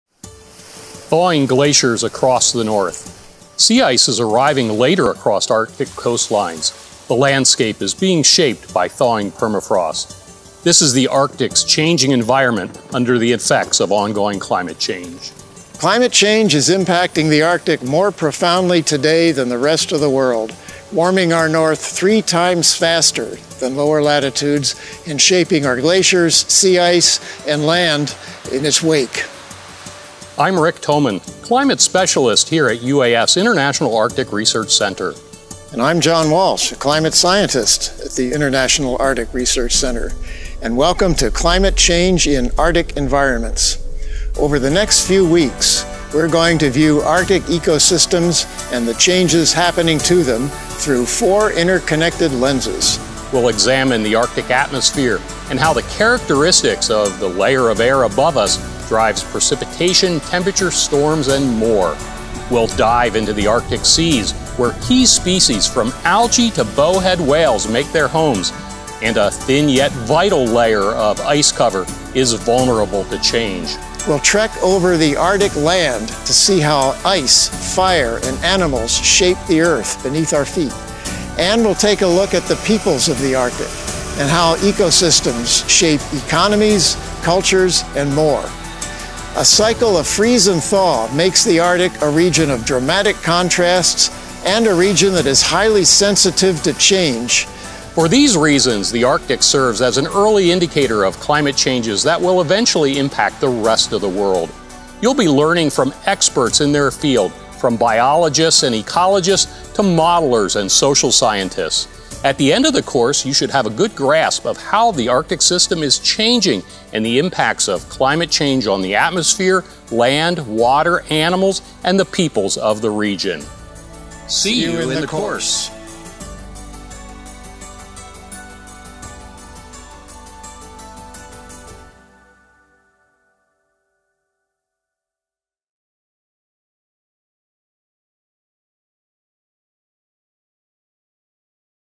Climate Change in Arctic Environments - About Video
Climate Change in Arctic Environments is an AlaskaX course led by Rick Thoman and John Walsh at the International Arctic Research Center. Learn from researchers and experts from across the University of Alaska Fairbanks, and around the world about our changing Arctic environment.